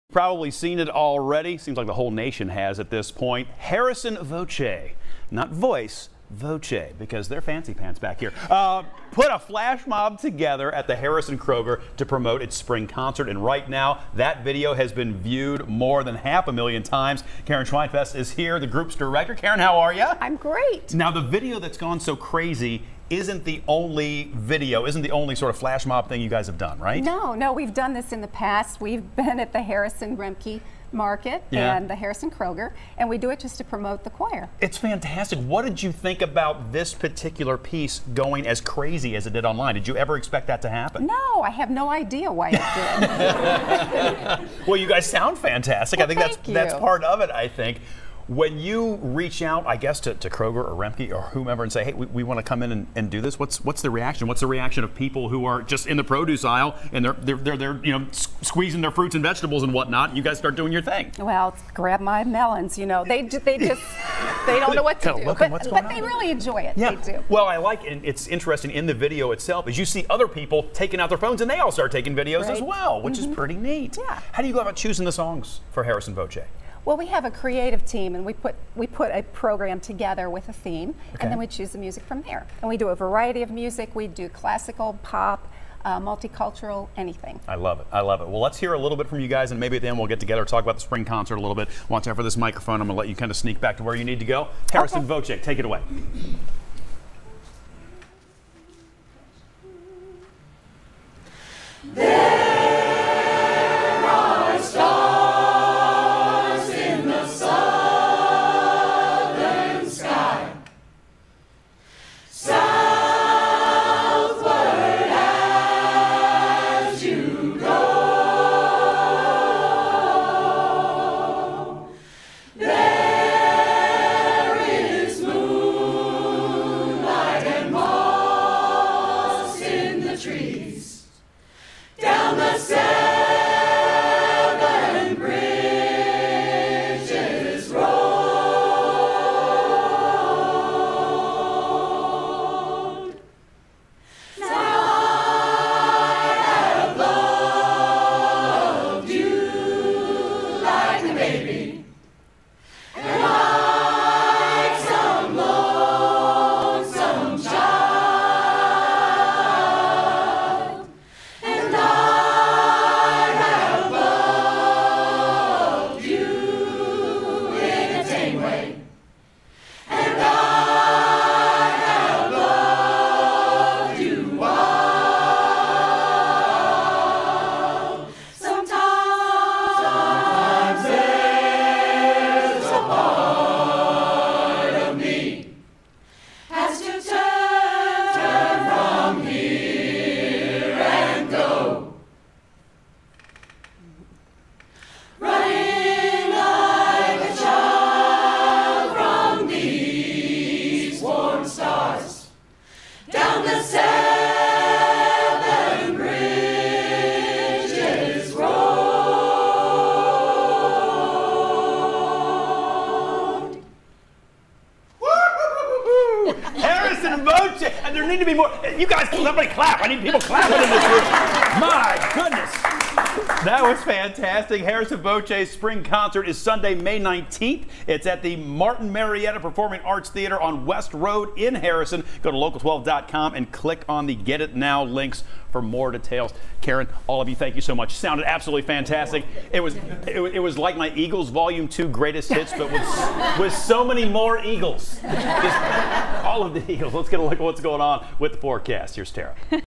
Performance from Harrison Voce ahead of their spring concert
CINCINNATI (WKRC) - Harrison Voce push a flash mob together at the Harrison Kroger to promote their spring concert. As of May 9, it has been viewed thousands of times. Heather Vest is the group's ringleader. The group has a concert May 19 at 4 p.m.